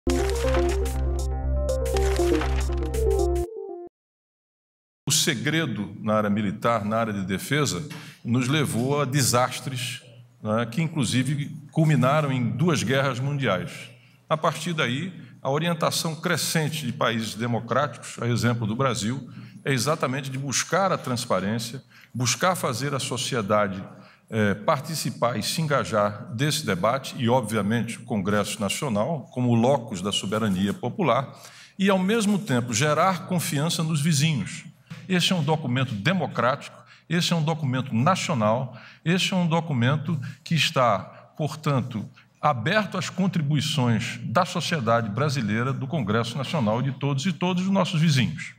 Ministro da Defesa ressalta transparência ao apresentar planejamento de ações das Forças Armadas
EM DESTAQUE - 29.09.16: O Ministério da Defesa apresentou ao presidente Michel Temer um balanço da atualização do planejamento das Forças Armadas para os próximos anos. Em entrevista coletiva, o ministro da pasta Raul Jungmann falou sobre a importância da transparência na apresentação desse planejamento.